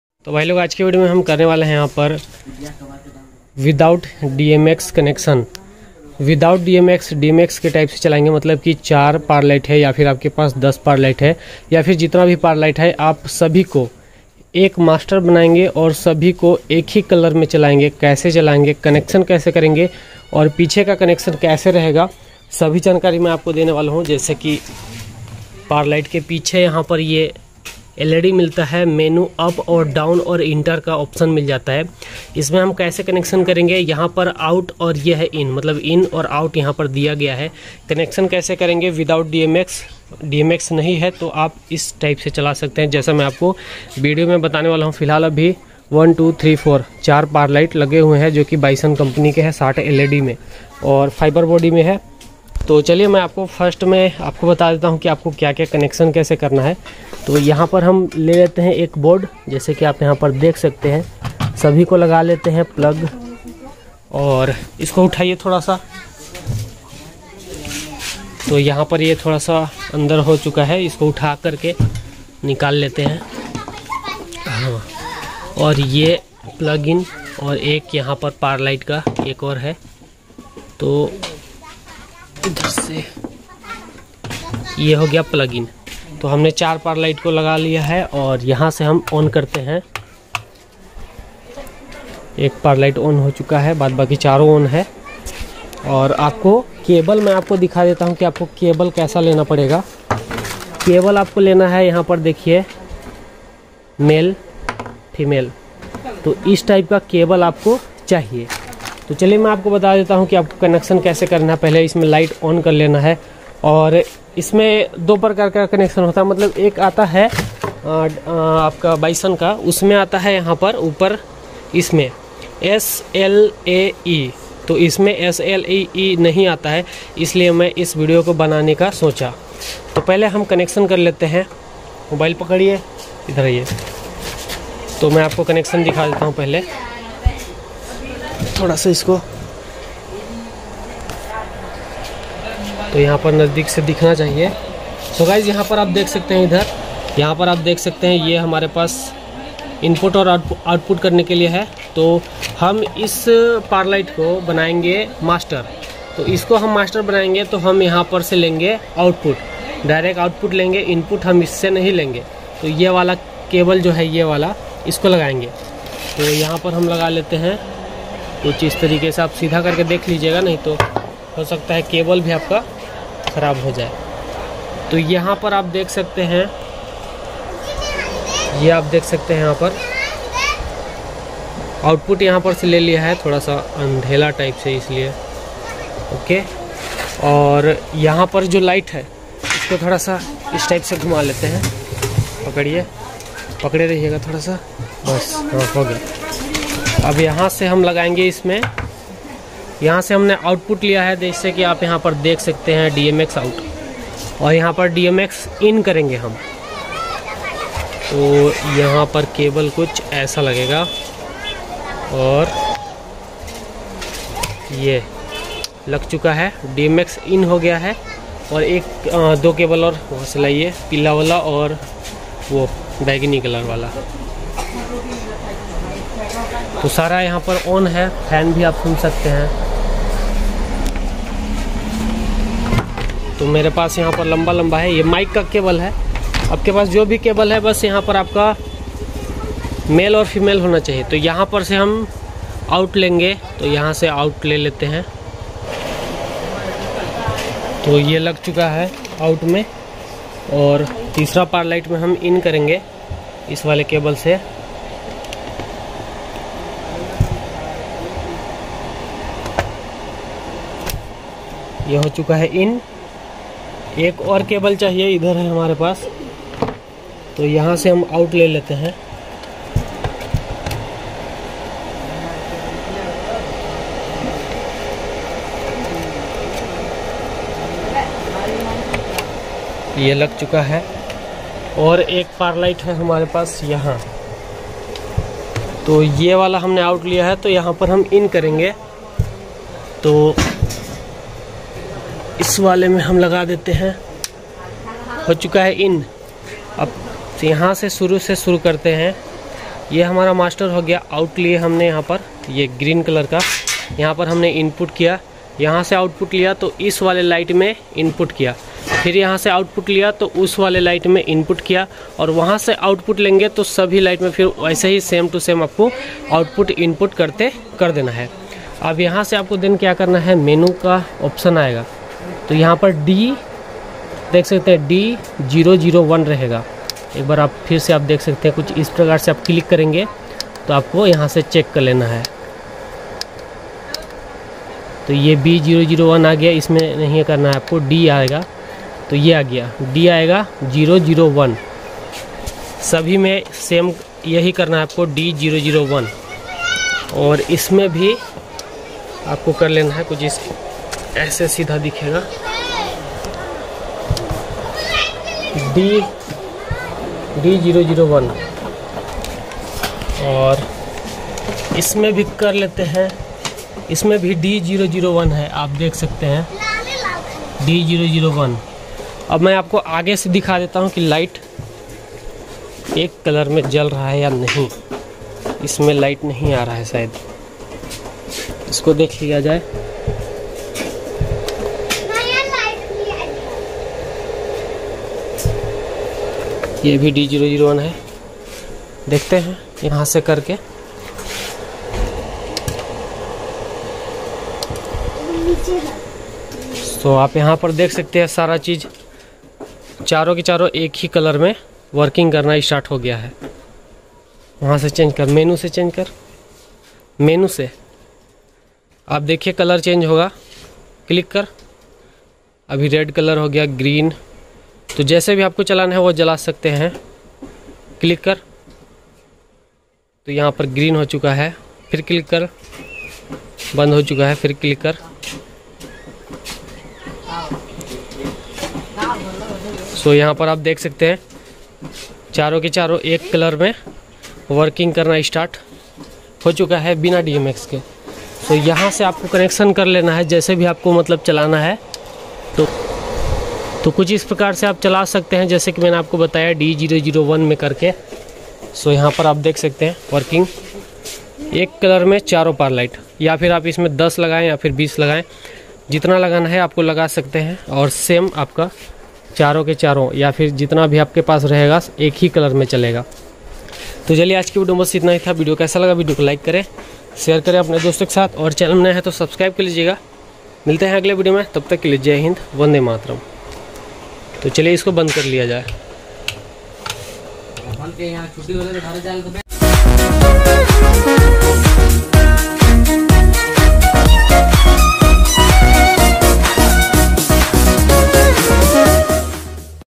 Bina Dmx Ke Par Light Kaise Chalay 🤔 Dmx की तरह कैसे चलेगा देखिये Setting | Dj Rock
Bina Dmx Ke Par Light Kaise Chalay Dekhiye Kaise Connection Kare

Es video me aapko bina dmx ke par light kaise chalaya jata hai dekhiye video